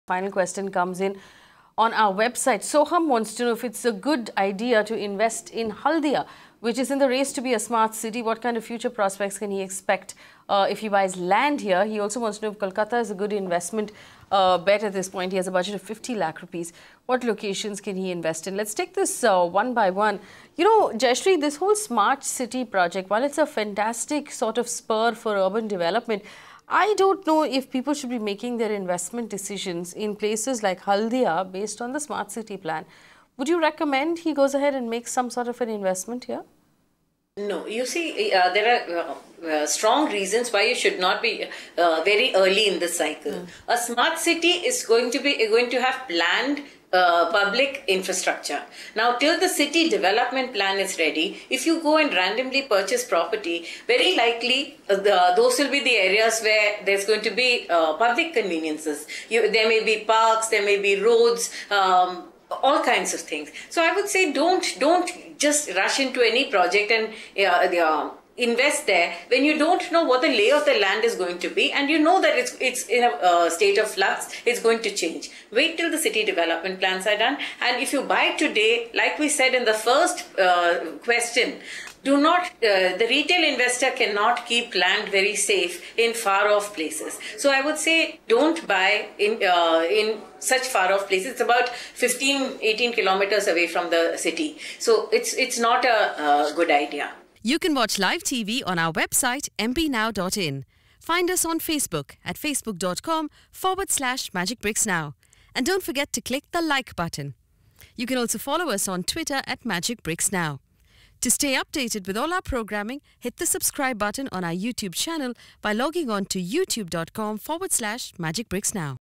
Is Kolkata a good investment bet?- Property Hotline
Expert: Jayashree Kurup, Head-Content & Research, Magicbricks.com
Question: Is it a good idea to invest in Haldia which is in the race to be a smart city? What kind of future prospects can I expect if I buy land here? Additionally, is Kolkata a good investment bet? My budget is Rs. 50 lakh. Please suggest good locations in Kolkata I can invest in.
Answer: A smart city is going to be a Planned Public Infrastructure. Till the city development plan is ready and if you randomly purchase the property, very likely those are the areas which are going to be for the public convenience.  Hence, unless you have a long term investment horizon, you should be investing close to the city, which is easily accessible and will witness appreciation in the near future.